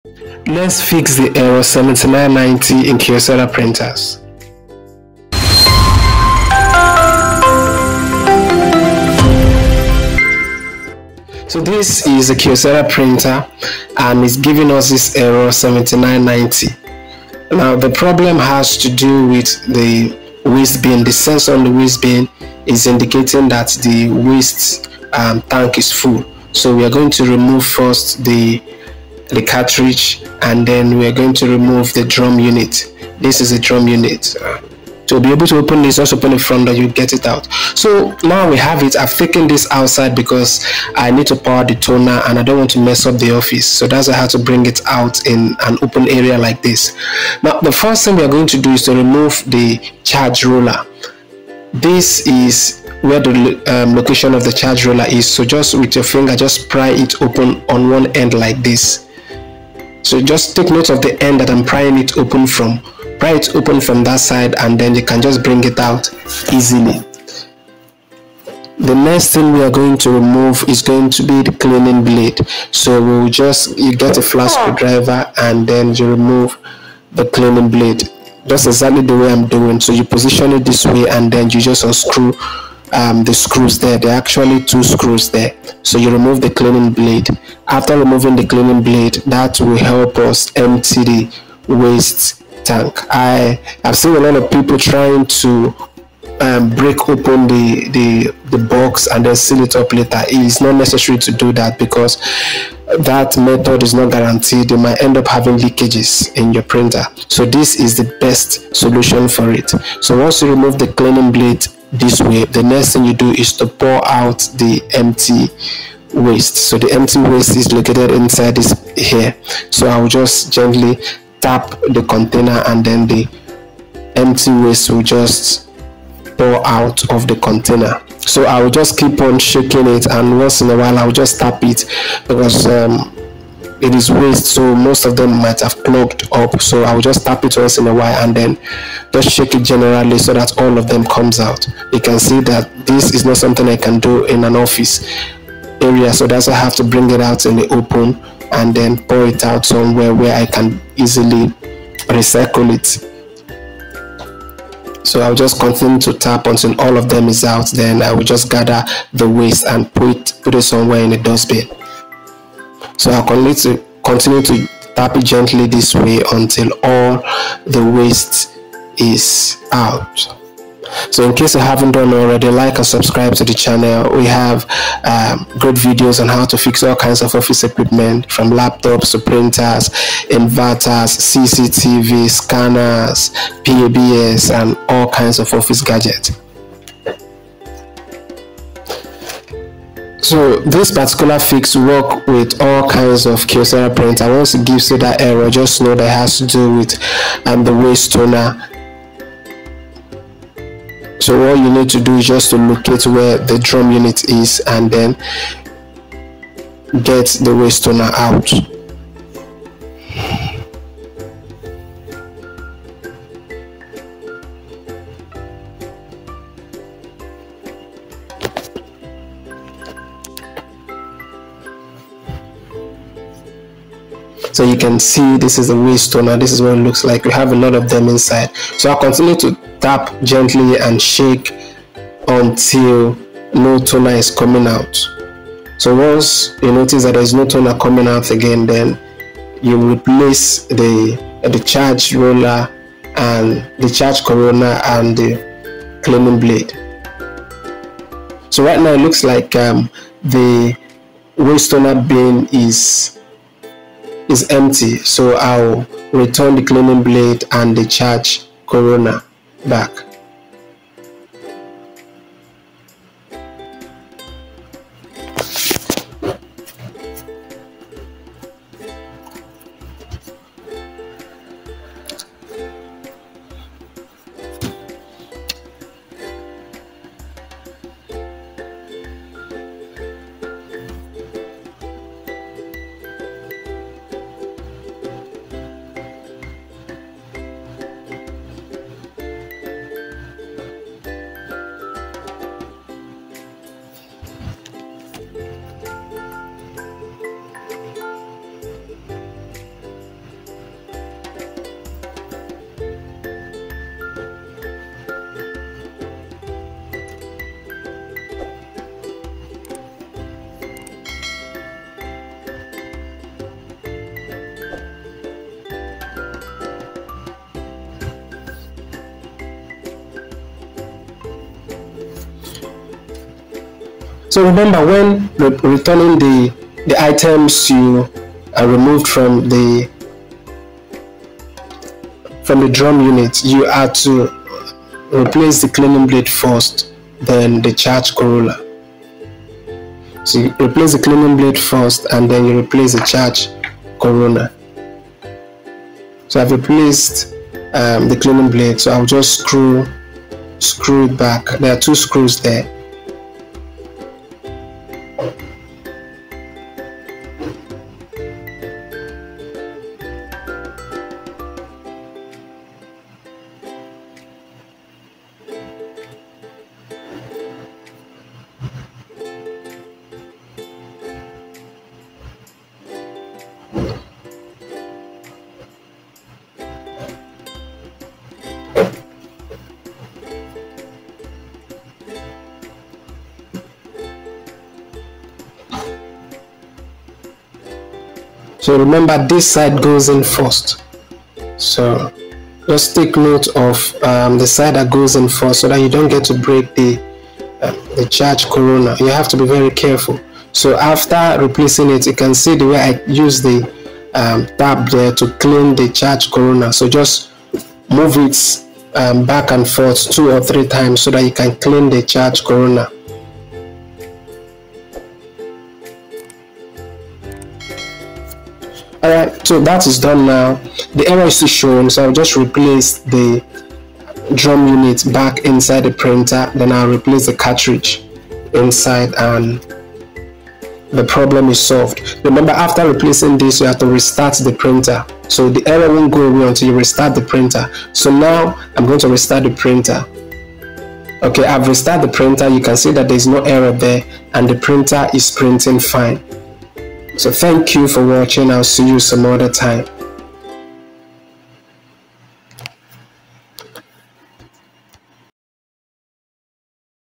Kyocera Call Service 7990 Error Fix
in this video i will show how to fix the call service 7990 Error in Kyocera printers